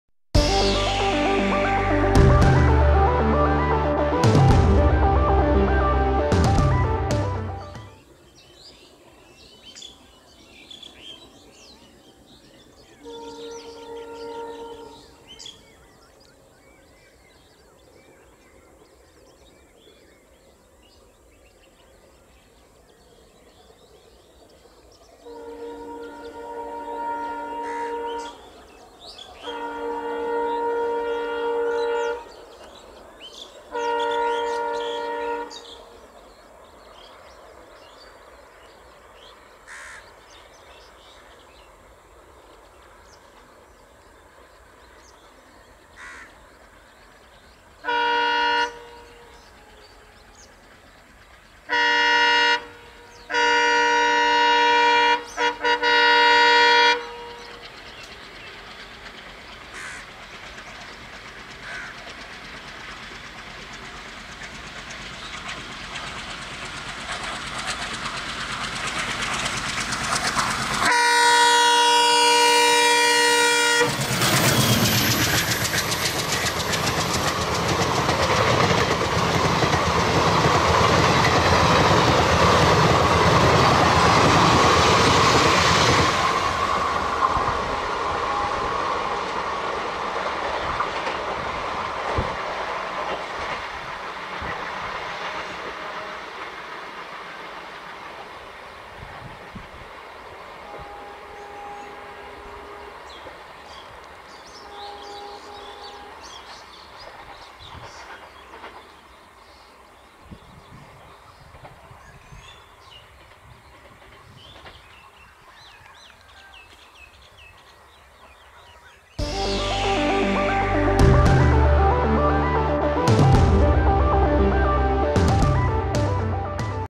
Malda Town Intercity Express are Accelerating in top speed after crossing a huge Curvy Rail Track
Howrah-Malda Town Intercity Express Train  (Via Azimganj)
Train no: 13465
Route:  Howrah Junction to Azimganj
Departure from Howrah junction  Railway Station: 15;15
Arrival at Azimganj Railway Station: 19:48
Running days: Daily
Available Classes: 2S CC
Train type: Mail/Express
Train Zone: ER / Eastern Railway
Travel distance: 218 km
Travel time: 4 hours & 33 min
Max permissible Speed: 100 km/hr
Average speed: 48 km/hr
Halt Stations: 7